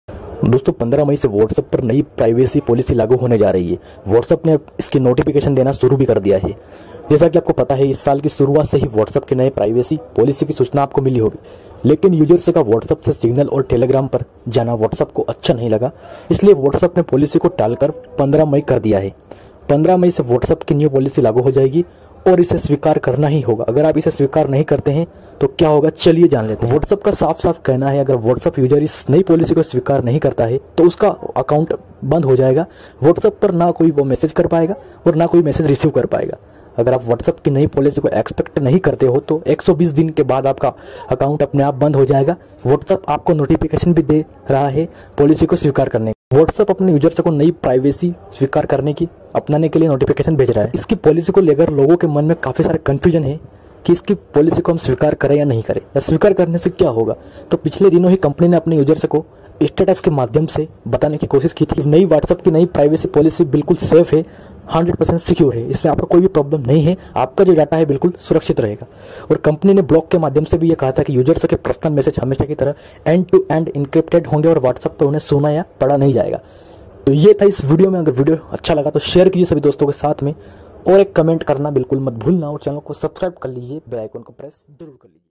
WhatsApp Policy Agree Nahi Kare To Kya Hoga - Whatsapp Privacy Policy Kya Hai | New Policy Update
WhatsApp Policy Agree Nahi Kare To Kya Hoga - Whatsapp Privacy Policy Kya Hai

Hello everyone is video me hamne bat ki hai agar aap whatsapp ki new policy 15 may accept nahi karenge to kya hoga is video me bataya hai.

Whatsapp privacy policy new update
Whatsapp new privacy policy again may 2021.

Whatsapp privacy policy kya hai

Whatsapp new policy accept kare ya nahi